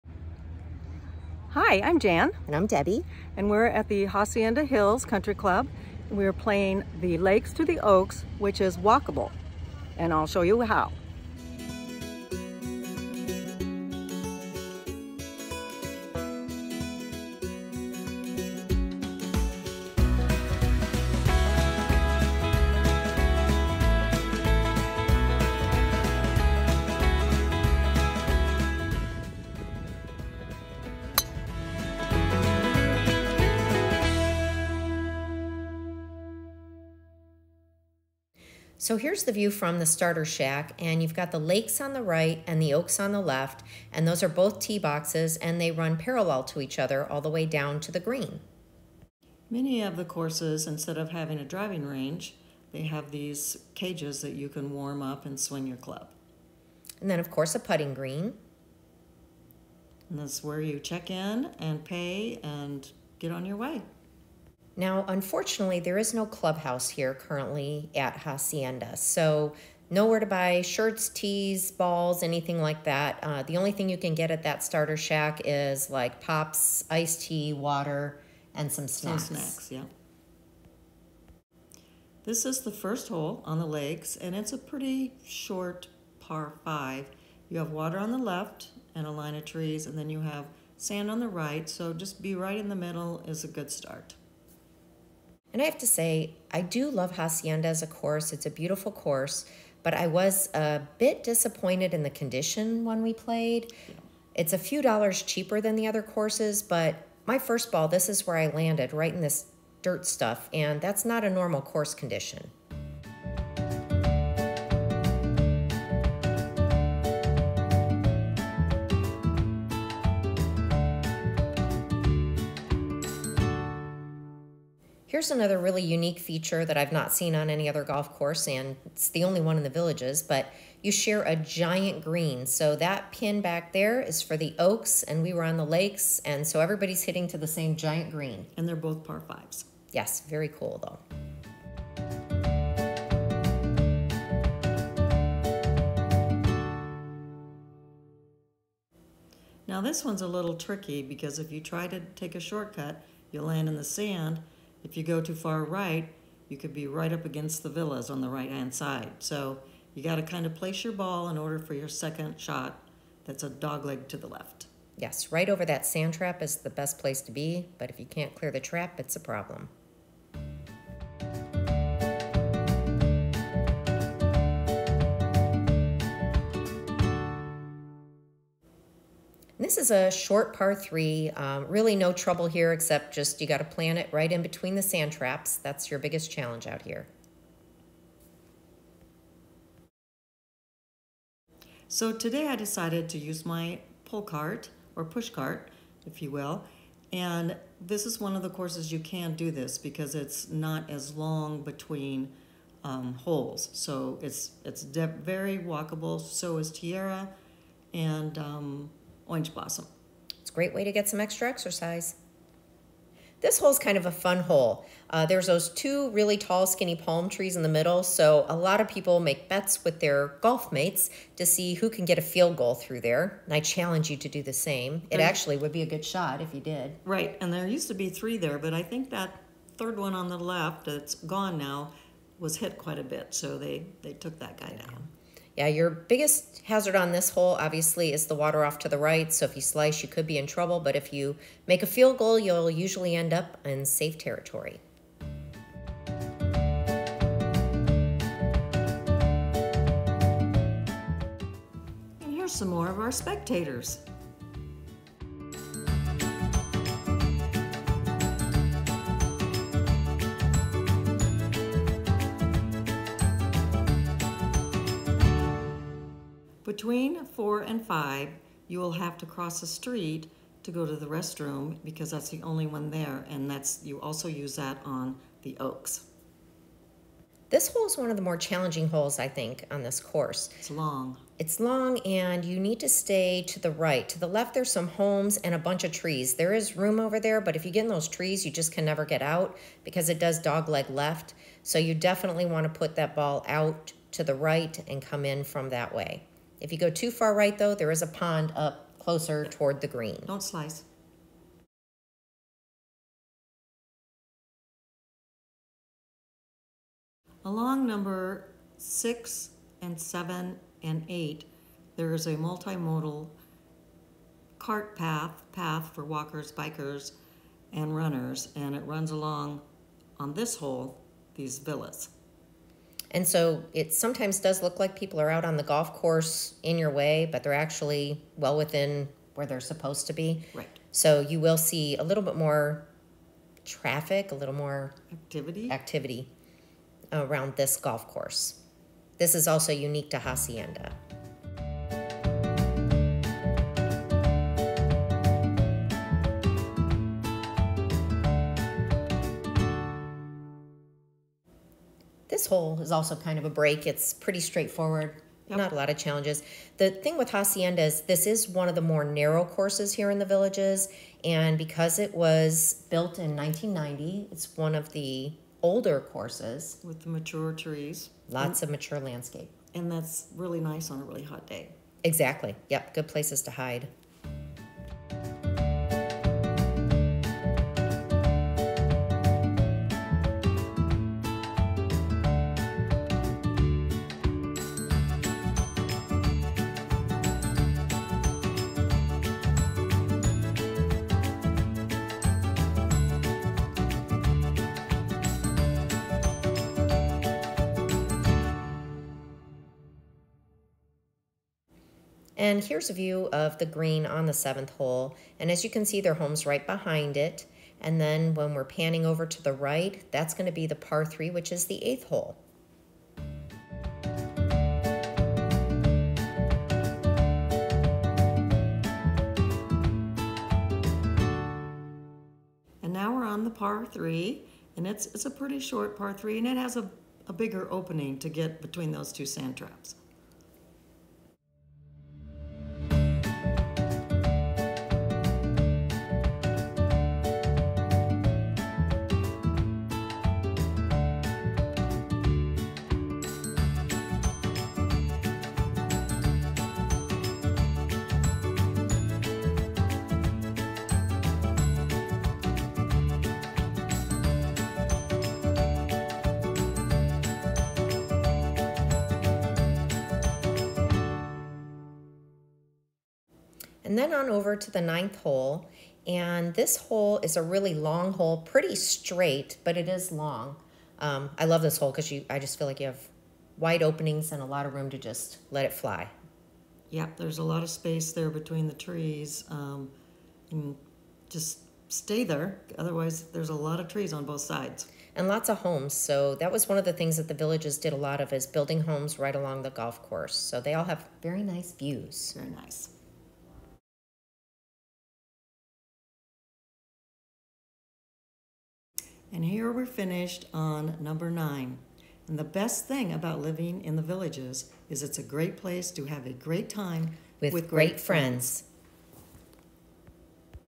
Golfing the SHORTEST 27 hole course Hacienda Hills in The Villages, FL - The Lakes
We golfed the Lakes/Oaks.   This is the Lakes course.   This combination of courses is just at 6400 yards.   The shortest of the 27 hole championship courses.